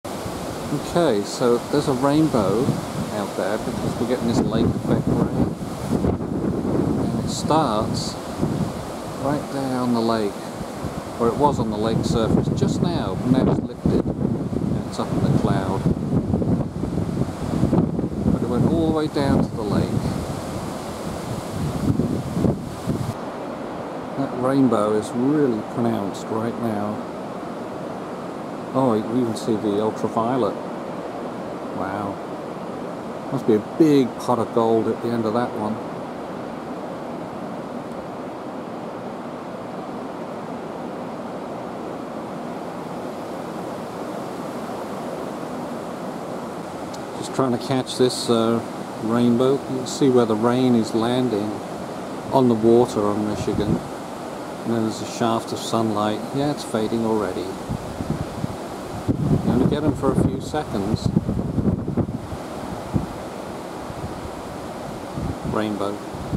MI 2024 Rainbow on Lake Michigan
These don't last long and the light needs to be just right.